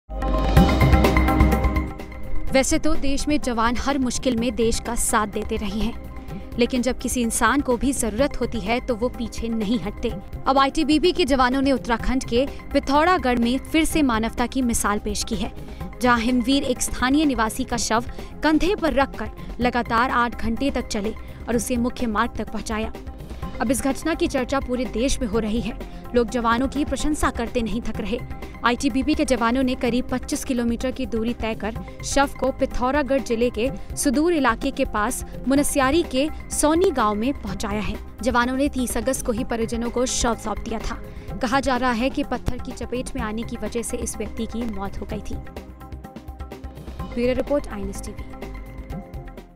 ITBP के जवानों ने मानवता की पेश की मिसाल
आईटीबीपी के जवानों ने उत्तराखंड के पिथौरागढ़ में फिर से मानवता की मिसाल पेश की है, जहां 'हिमवीर' एक स्थानीय निवासी का शव कंधे पर रखकर लगातार आठ घंटे तक चले और उसे मुख्य मार्ग तक पहुंचाया.